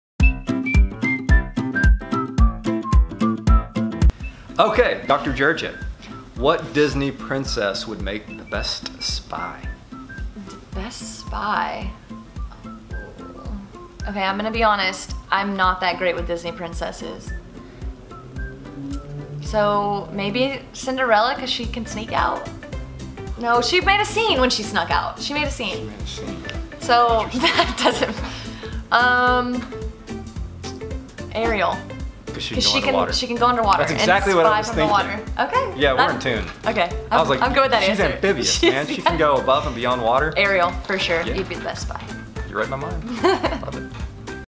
RQs with Dr. Smith & Dr. Juricek | Episode 4
We put both Dr. Smith and Dr. Jurieck on the spot with total random questions without any notice of what the question will be. We hope you guys enjoy it because we certainly did.

Dr. Smith | Dr. Juricek
Breathe Modern Dentistry